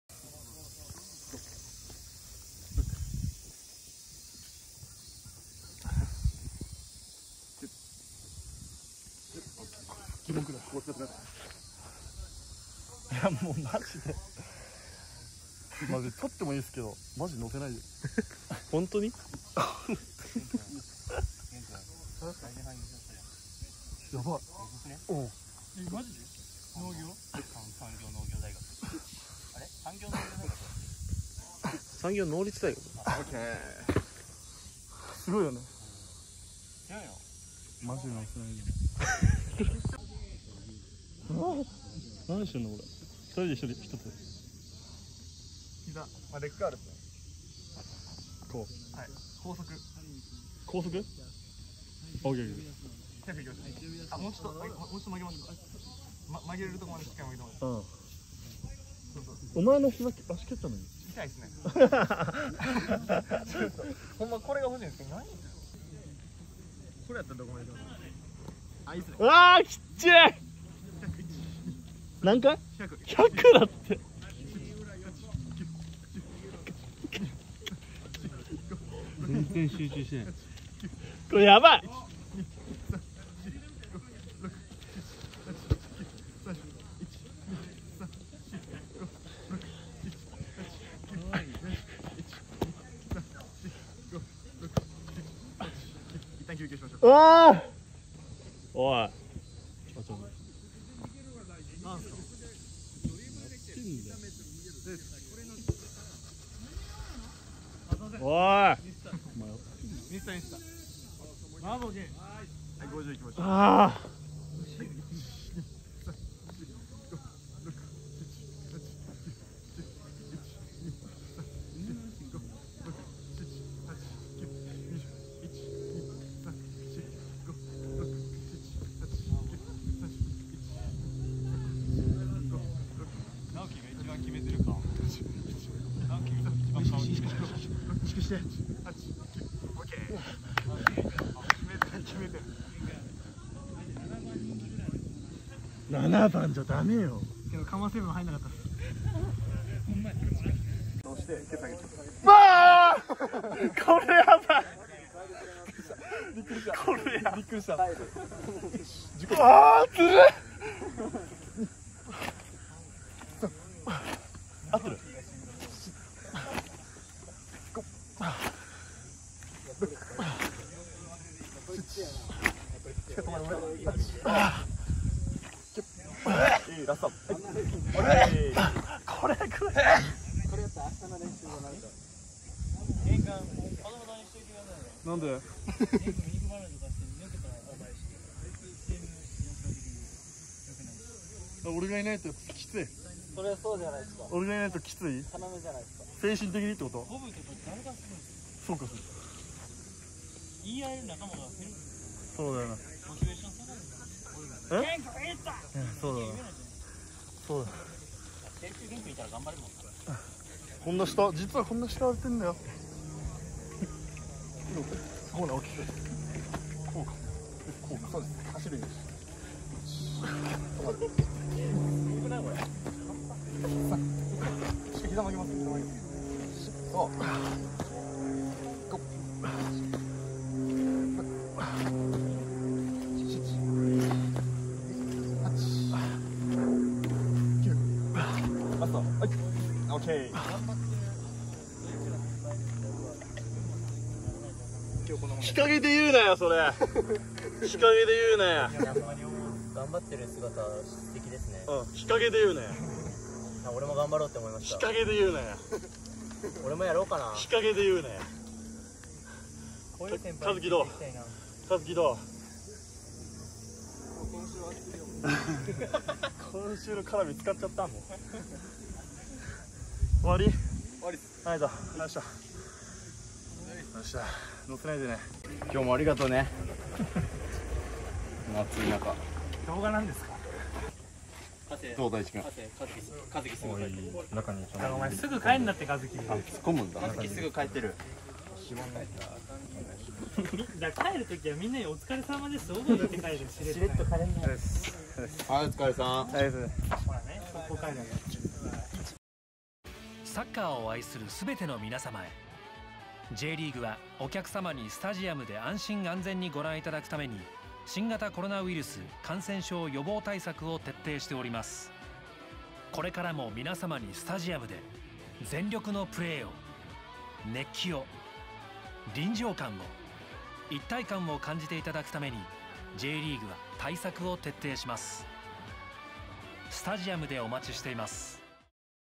【トレーニング動画】遠藤元一選手、鍛えてます！
全体練習終了後に、遠藤元一選手の自主練習を撮影
みんなから慕われている遠藤選手。薩川選手、川﨑選手らも登場